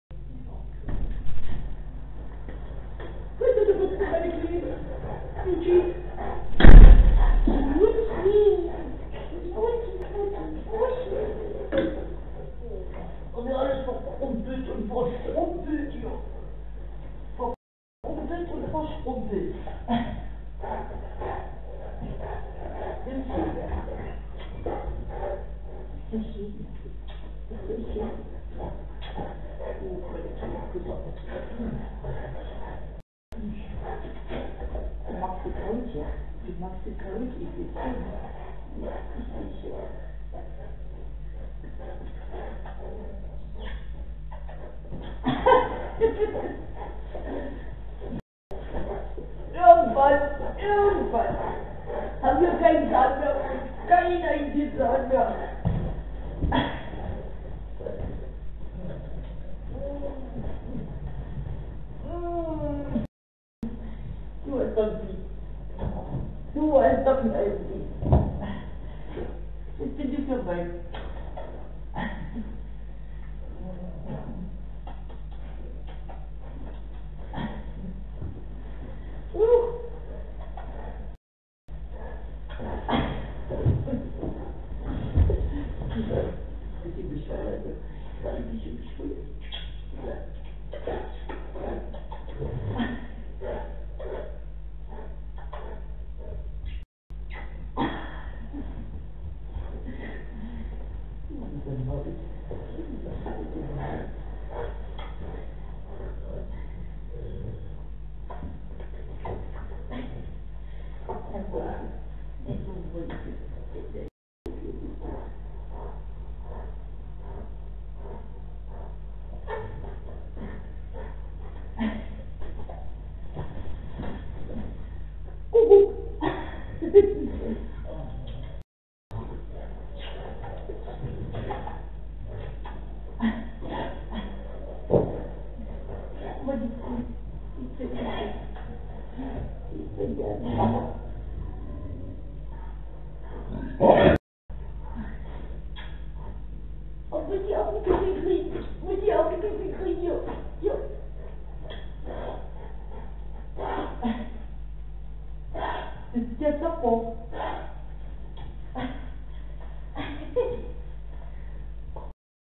Bernies die sanften Riesen Fortsetzung 🐶 🐶
Brita the Best. Hier mal ein Ausschnitt aus dem Berniezimmer.

Obwohl Bernhardiner sehr große Hunde sind kann man hier schön sehen wie sanft und umgänglich sie doch sind.